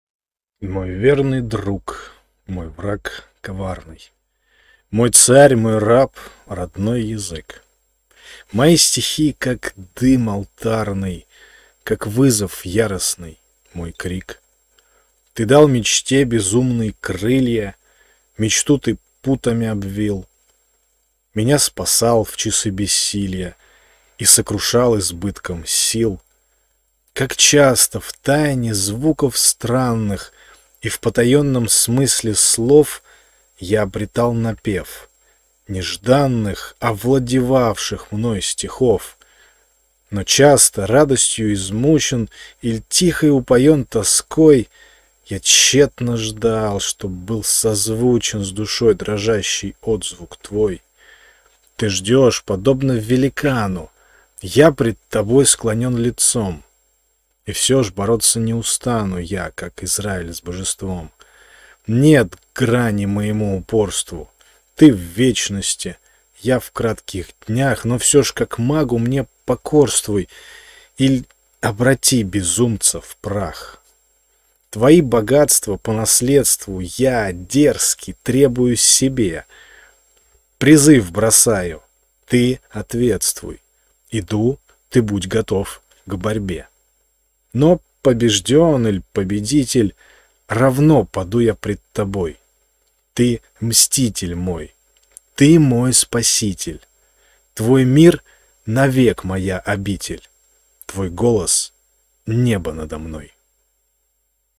Стихи // Родной язык // Валерий Брюсов
Мой верный друг! мой враг коварный!
Мой царь! мой раб! родной язык!
Мои стихи — как дым алтарный!
Как вызов яростный — мой крик!
Ты дал мечте безумной крылья,
Мечту ты путами обвил,
Меня спасал в часы бессилья
И сокрушал избытком сил.
Как часто в тайне звуков странных
И в потаенном смысле слов
Я обретал напев — нежданных,
Овладевавших мной стихов!
Но часто, радостью измучен
Иль тихой упоен тоской,
Я тщетно ждал, чтоб был созвучен
С душой дрожащей — отзвук твой!
Ты ждешь, подобен великану.
Я пред тобой склонен лицом.
И всё ж бороться не устану
Я, как Израиль с божеством!
Нет грани моему упорству.
Ты — в вечности, я — в кратких днях,
Но всё ж, как магу, мне покорствуй,
Иль обрати безумца в прах!
Твои богатства, по наследству,
Я, дерзкий, требую себе.
Призыв бросаю, — ты ответствуй,
Иду, — ты будь готов к борьбе!
Но, побежден иль победитель,
Равно паду я пред тобой:
Ты — Мститель мой, ты — мой Спаситель,
Твой мир — навек моя обитель,
Твой голос — небо надо мной!

31 декабря 1911

стихи, поэзия, poetry, поэт, чтец, серебряный век, Брюсов, Валерий Брюсов